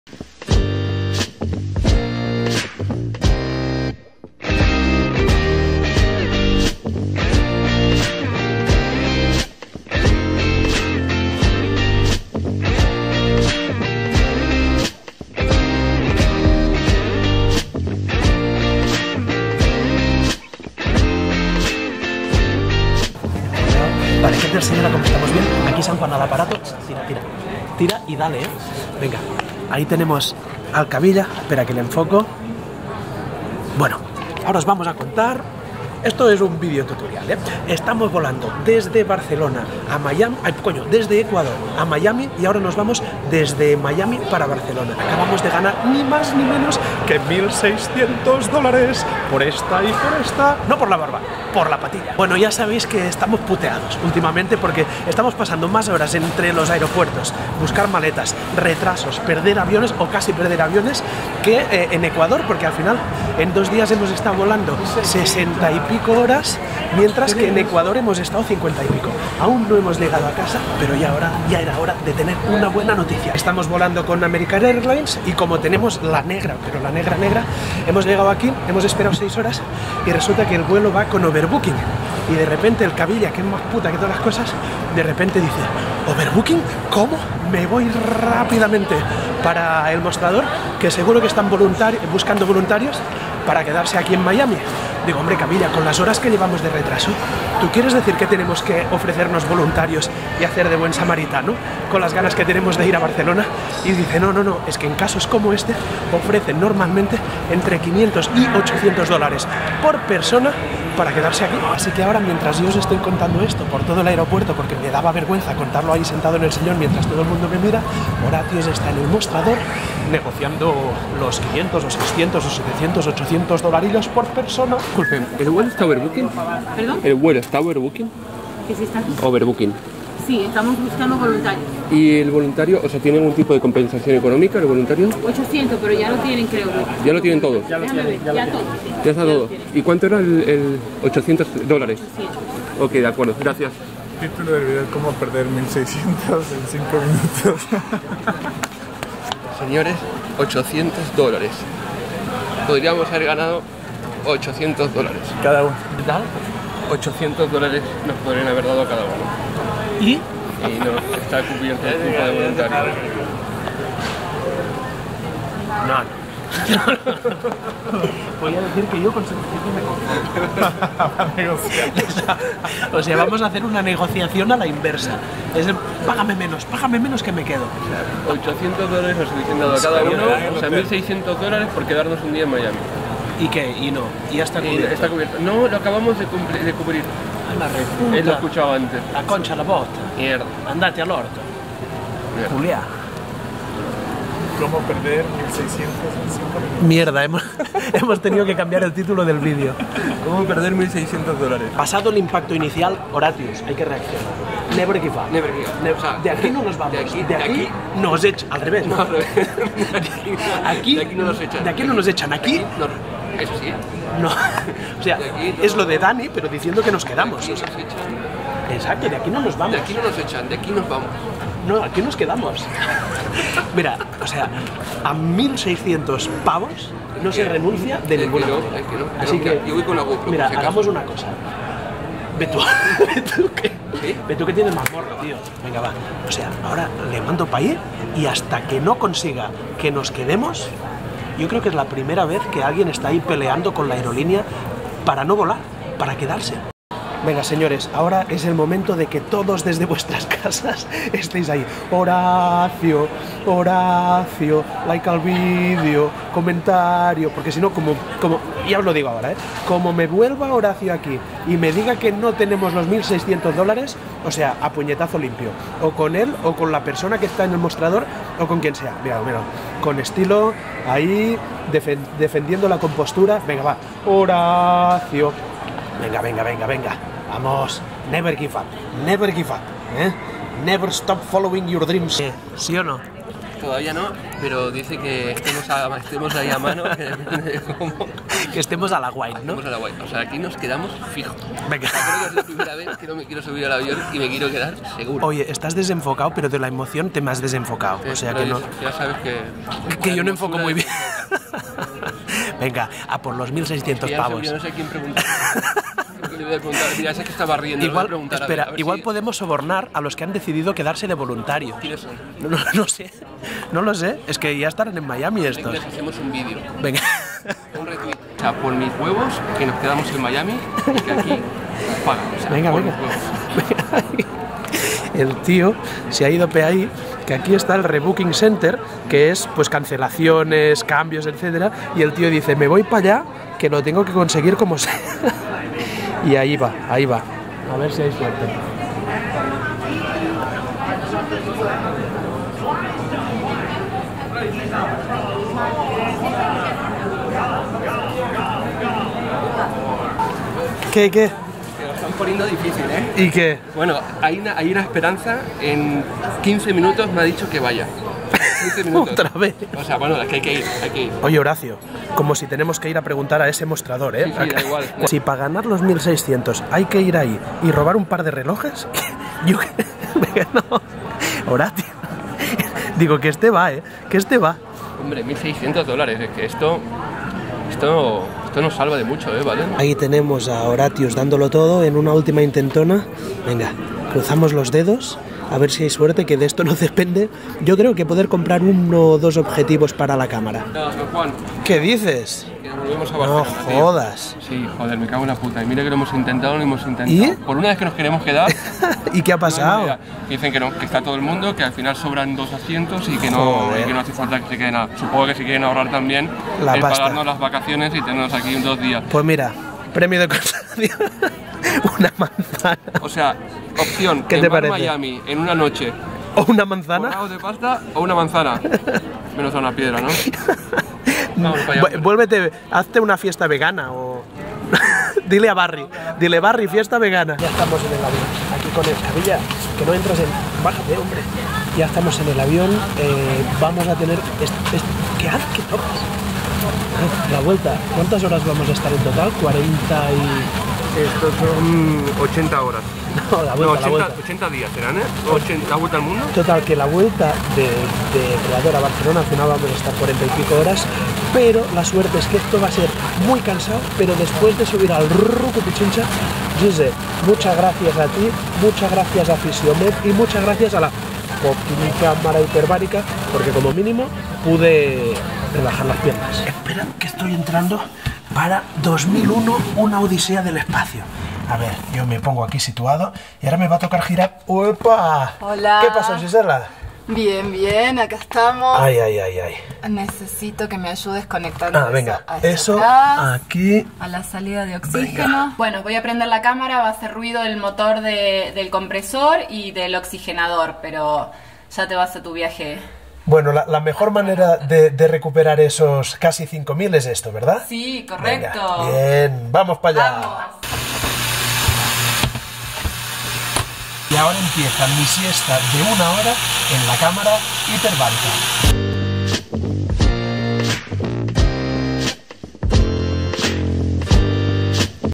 Cómo Perdimos 1.600$ en 10 Minutos
Cuando uno tiene mala suerte, tiene mala suerte. Y parece que Horatius y yo esta semana tenemos la negra.